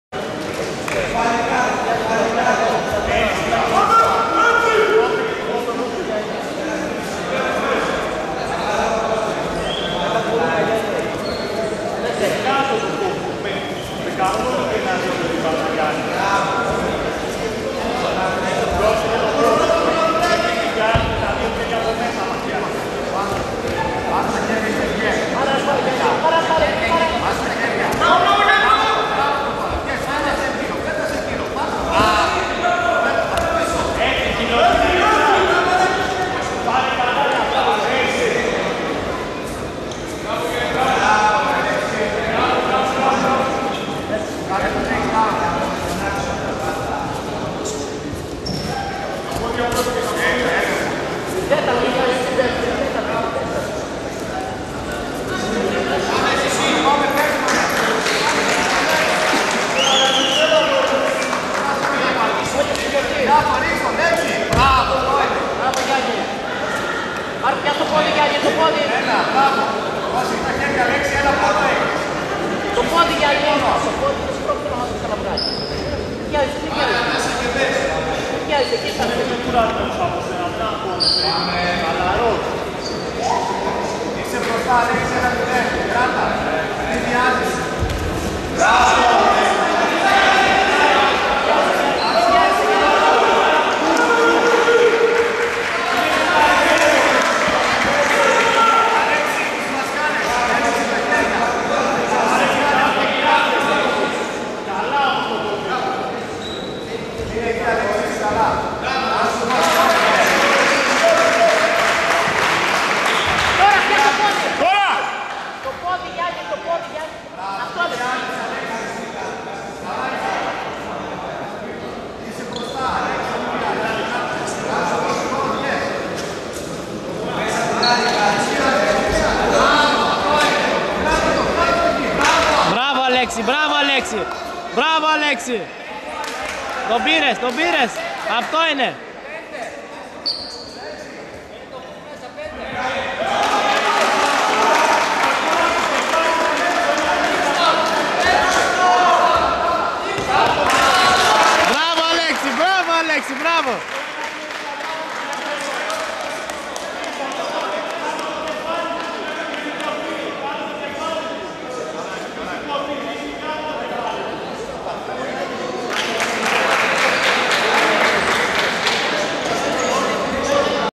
ALEX(GOUGOULAS TEAM)IN SHOOTO GRAPPLING ATHENS -83 BEGINERS
ALEX FROM GOUGOULAS TEAM IN SHOOTO GRAPPLING ATHENS FIGHT 1 WIN BY SUBMISSION